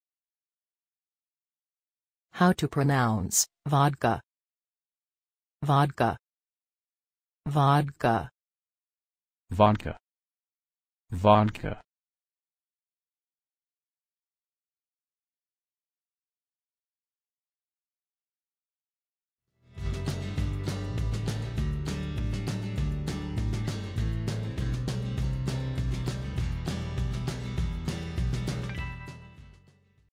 How to say Vodka
How to pronounce Vodka in English? Listen with us.
What is the correct pronunciation of the word Vodka in everyday English? Speak as the Americans.
How to write the word Vodka? Watch and learn.

Learn how to pronounce Vodka.